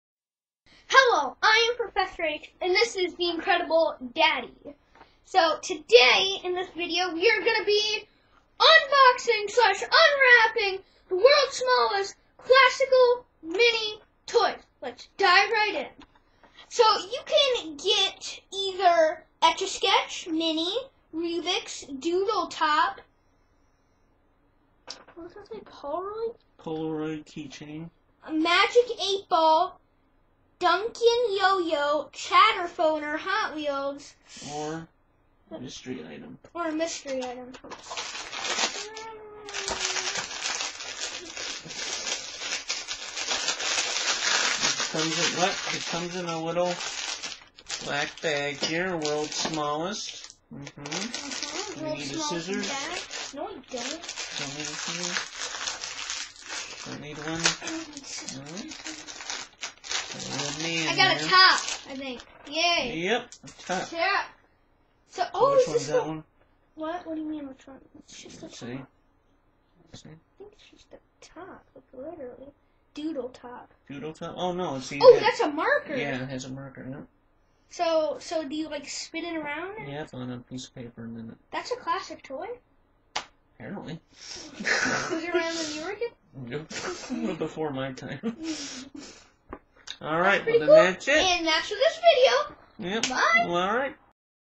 World's Smallest Minitoy Unboxing! Classic Mini Collectable toys!
Join us in Unboxing this Mystery Secret Smallest box featuring the World's Smallest toys that actually work!